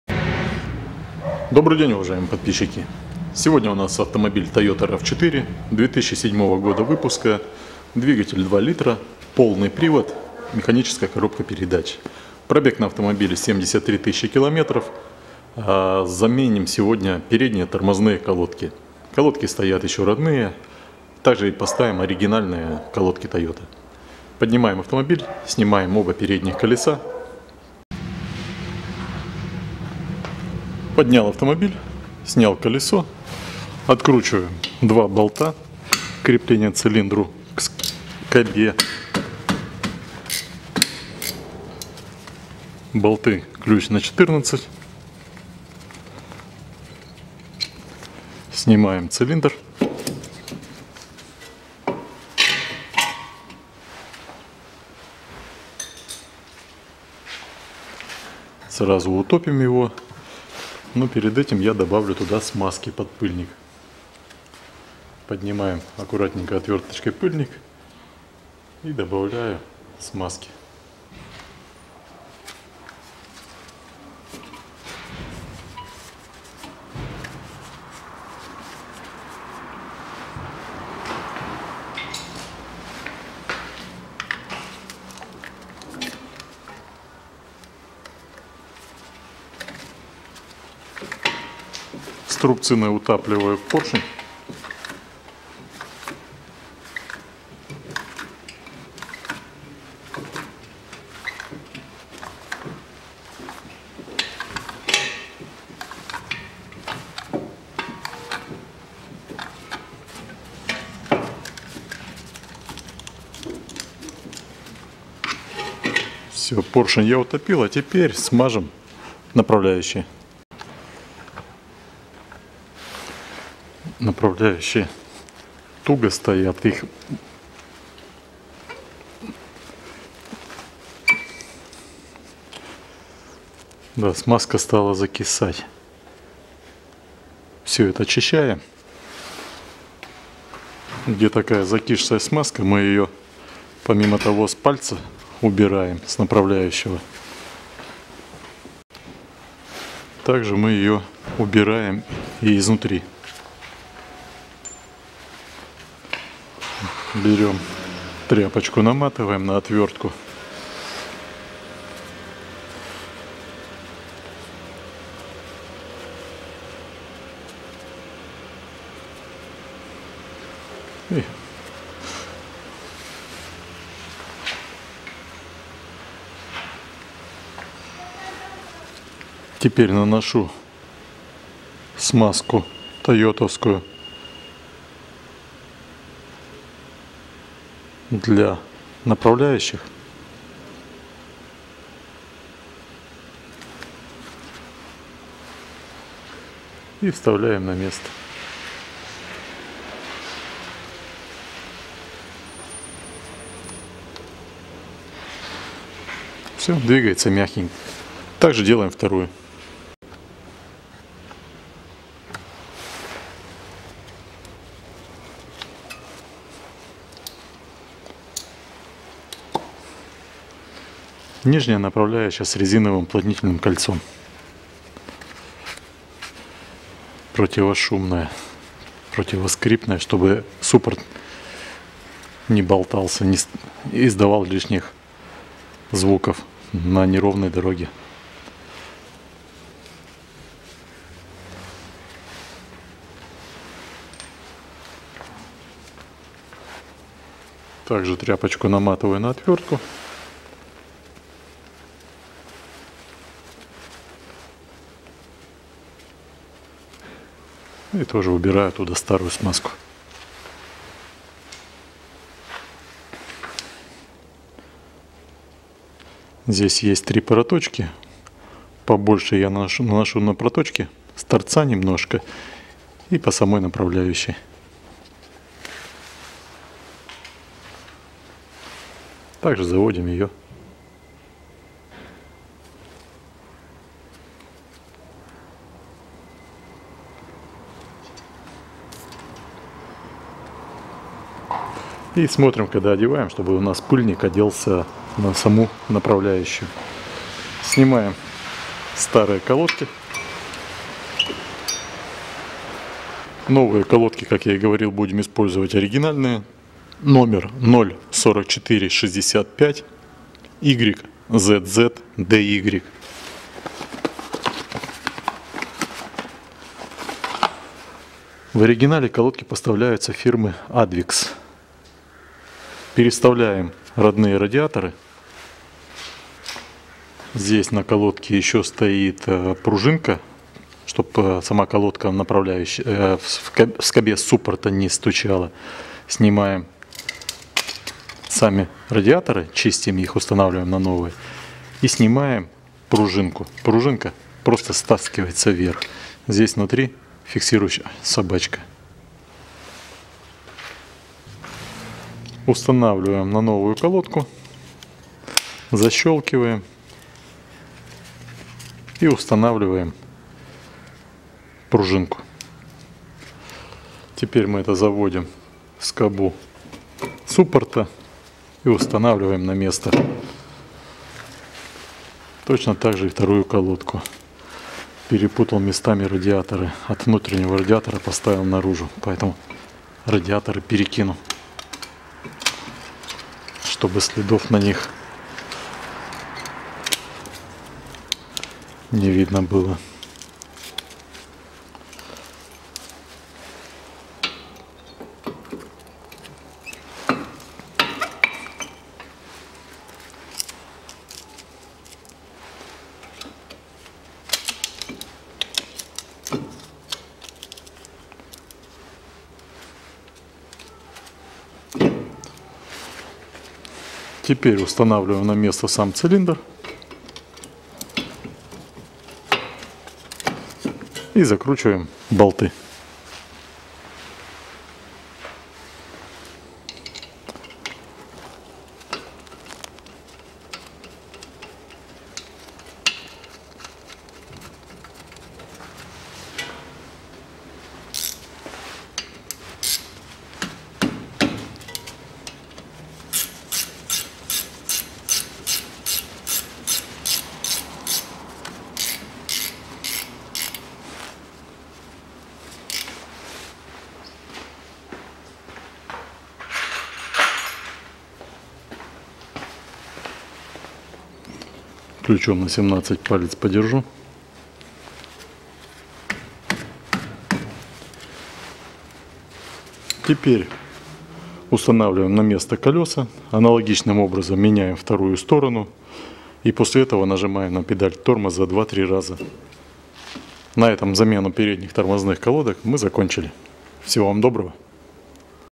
Замена передних тормозных колодок Toyota RAV4 2,0 Тойота РАВ 4 2007 года
колодки тормозные  04465-YZZDY